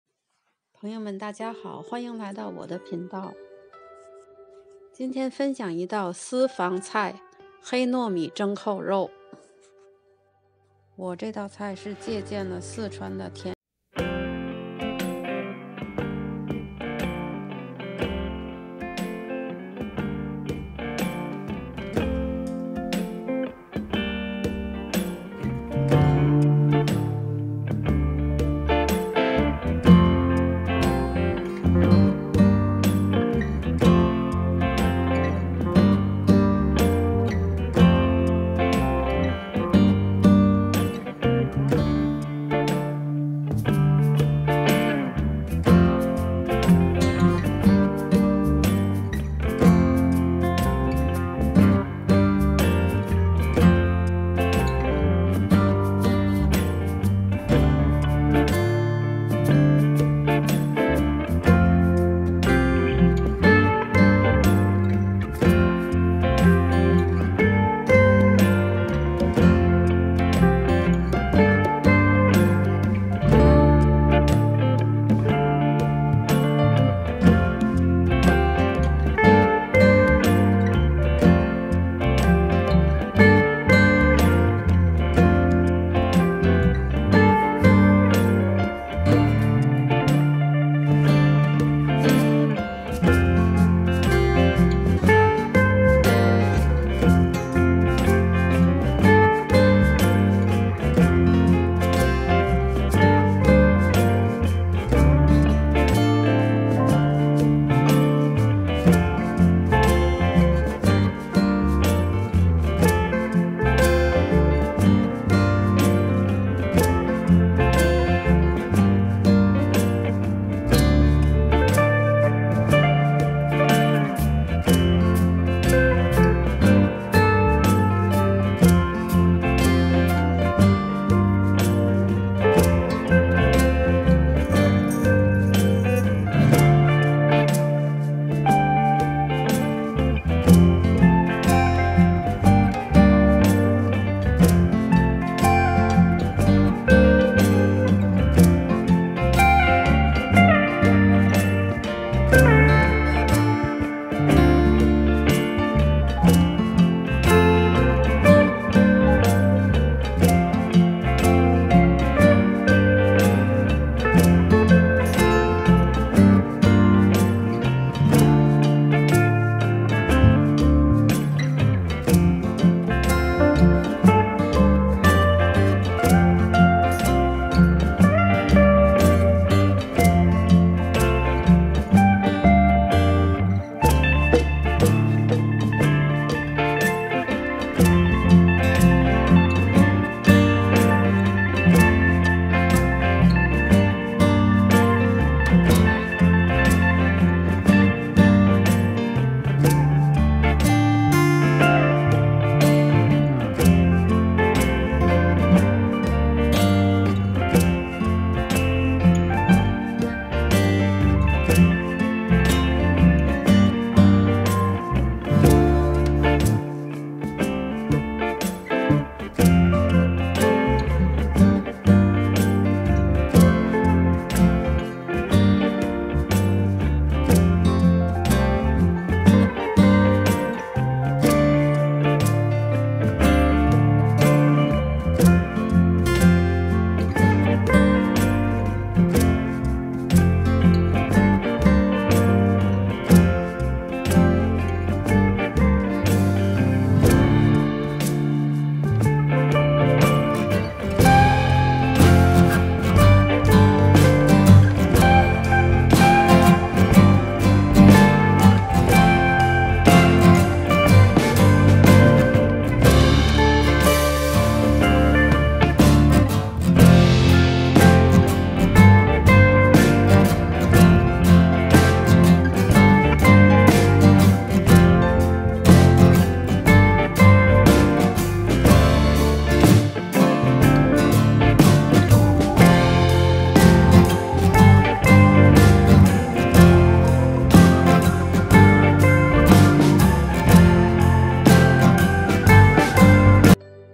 黑糯米扣肉，甜烧白与咸烧白美结合 Steamed pork with black glutinous rice|Chinese cuisine
黑糯米扣肉，甜烧白与咸烧白的完美结合
黑糯米具有抗氧化，补血养气，富含多种微量元素 例如钙，磷，铁，B1，B2，而且蛋白质，脂肪的含量也丰富，多吃深颜色的食物对身体有益。

黑糯米蒸肉，是甜烧白与咸烧白的结合，黑糯米代替了白糯米，增加了健康元素，米吃起来弹牙，肉香糯。

主要材料：
猪肉1磅，黑糯米1杯
调味料：生抽1大匙，老抽1/2大匙，蚝油1大匙，盐1小茶匙，糖1大茶匙，料酒或米酒1大匙

main ingredients:
pork 1 LB, black stick rice 1 cup
sauce: light soy sauce 1 TBS, dark soy sauce 1/2 TBS, oyster sauce 1 TBS, salt 1 tsp, sugar 1TBS, cooking wine 1 TBS
steamed pork belly
steamed pork meat
steam meat with glutinous rice